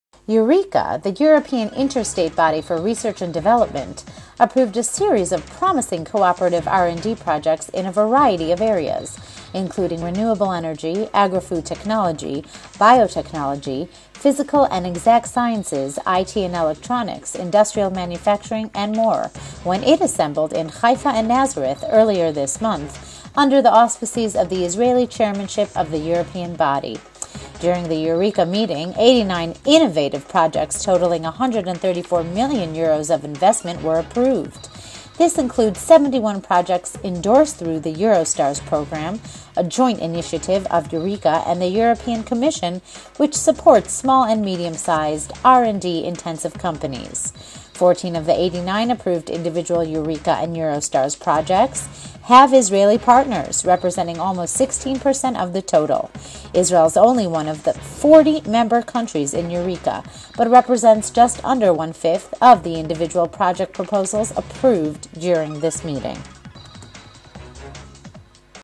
Eureka will invest in 14 projects
the European inter-state body for Research and Development - approved a series of promising cooperative R&D projects in a variety of areas, including  renewable energy, agrofood technology, biotechnology, physical and exact sciences, IT and electronics, industrial manufacturing, and more when it assembled in Haifa and Nazareth earlier this month, under the auspices of the Israeli Chairmanship of the European body.
During the EUREKA meeting, 89 innovative projects totaling 134 million Euros of investment were approved.  This includes 71 projects endorsed through the Eurostars program, a joint initiative of EUREKA and the European Commission which supports small and medium sized R&D intensive companies.
Fourteen of the 89 approved individual EUREKA and Eurostars projects have Israeli partners  -- representing almost 16% of the total. Israel is only one of 40 member countries in EUREKA but represents just under one fifth of the individual project proposals approved during this meeting.